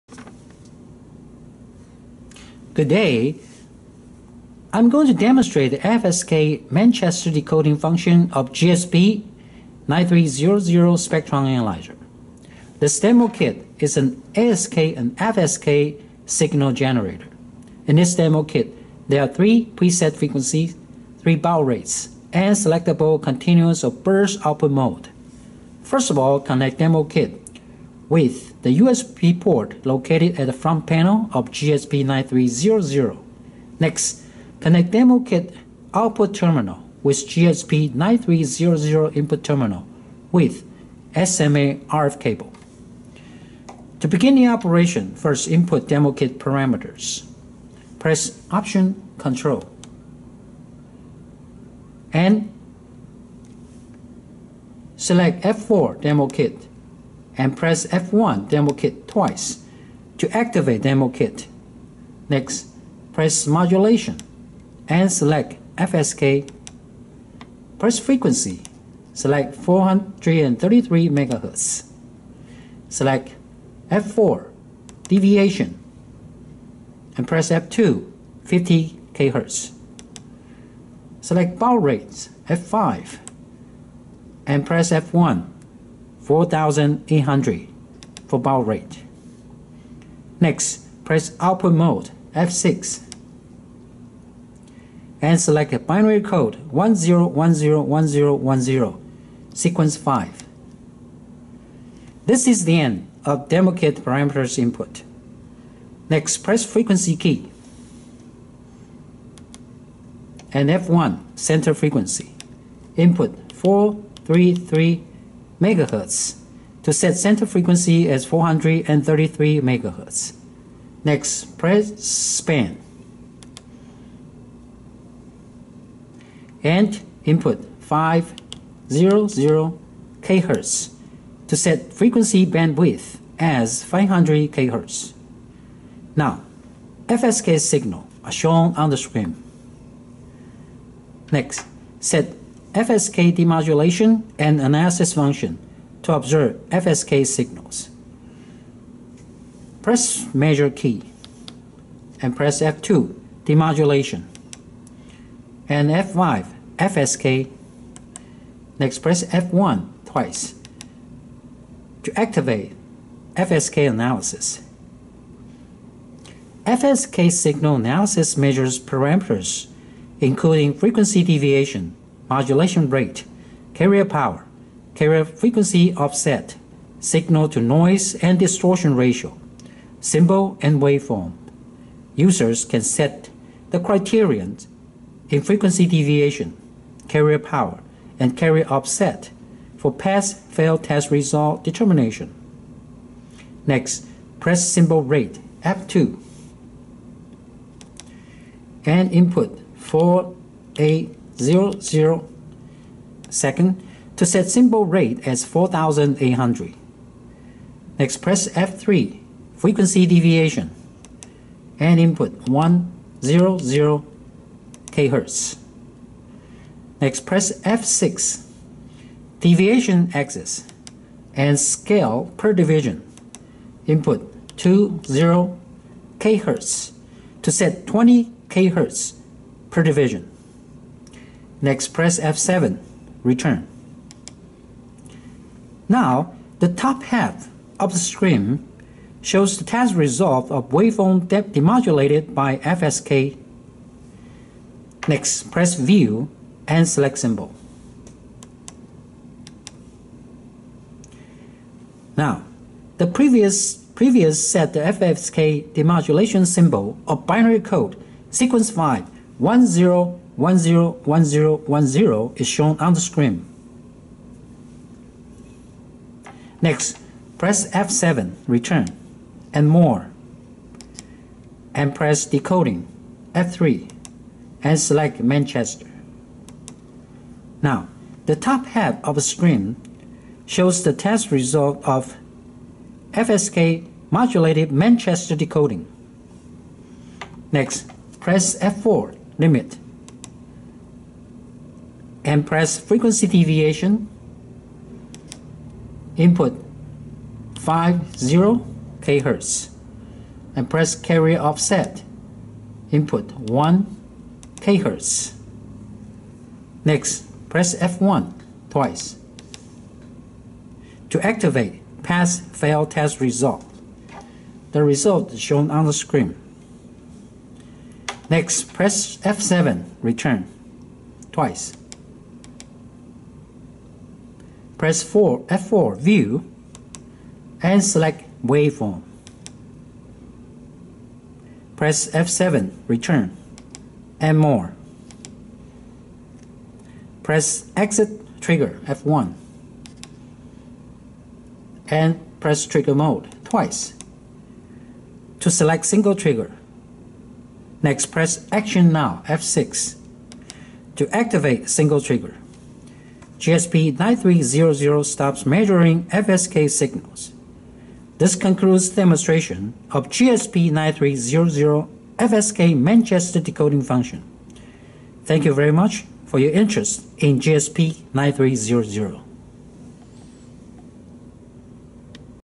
GSP-9300 FSK Manchester Decoding Function
Now, I am going to demonstrate the FSK Manchester Decoding Function of GSP-9300 spectrum analyzer.
This demo kit is an ASK and FSK signal generator. In this demo kit, there are three preset frequency, three Baud rates and selectable continuous or burst output mode. First of all, connect demo kit with the USB port located at the front panel of GSP-9300. Next, connect demo kit output terminal with GSP-9300 input terminal with SMA RF cable.

1. To begin the operation, first, input demo kit parameters.
2. Press option control key, demo kit (F4) key, demo kit (F1) key twice to activate demo kit
3. Press modulation (F2) key and select FSK (F2) key
4. Press frequency (F3) key and select 433 MHz (F2) key
5. Press deviation (F4) key and select frequency deviation of 50kHz (F2) key
6. Press Baud rate (F5) key and select baud rate of 4800 (F1) key
7. Press output mode (F6) key and select binary code 10101010,Seq5(F7)
8. This is the end of demo kit parameters input.
9. Press frequency key, center freq (F1) key, input 4, 3, 3, MHz (F2) key to set center frequency as 433 MHz.
10. Press span key, span (F1) key, input 5, 0, 0, kHz (F3) key to set frequency bandwidth as 500kHz.
11. Now, FSK signals are shown on the screen.
12. Next, set FSK demodulation and analysis function to observe FSK signals.
13. Press measure key, Demod (F2) key, FSK Analysis (F5) , FSK Analysis (F1)twice to activate FSK analysis function.
14. FSK Signal Analysis measures parameters including frequency deviation, modulation rate, carrier power, carrier frequency offset, Signal-to-noise and distortion ratio (SINAD), symbol and waveform. Users can set the criterion in frequency deviation, carrier power and carrier offset for pass/fail test result determination.
15. Press symbol rate(F2) key, input 4,8,0,0 s (F2) to set symbol rate as 4800
16. Press Freq. Deviation(F3), input 100kHz
17. Press Deviation Axis (F6) key, and Scale/Div(F3), input 2,0 kHz(F3) to set 20kHz per div
18. Press return (F7) key
19. Now, the top half of the screen shows the test result of waveform demodulated by FSK. Next press View (F4) and select symbol (F1)
20. Now, the previous set FSK demodulation symbol of binary code Seq5,10101010 is shown on the screen
21. Press Return(F7), and More 1/2 (F7), press Decoding(F3), press Manchester (F3)
22. Now, the top half of the screen shows the test result of FSK modulated Manchester Decoding
23. Press Limit(F4)
24. Press Freq. Deviation(F2),input 5,0,kHz(F3)
25. Press Carrier offset(F3), input 1,kHz(F3)
26. Press Limit(F1) twice to activate pass/fail test result, press Return(F7) twice
27. Press View(F4), select waveform(F2), press Return(F7), and More 1/2(F7)
28. Press Ext Trigger (F1), Trigger mode (F3) twice, to set Single Trigger
29. Press Action Now (F6) to activate Single Trigger. GSP-9300 stops measuring FSK signals.
30. This concludes the demonstration of GSP-9300 FSK Manchester Decoding Function.
31. Thank you very much for your interest in GSP-9300.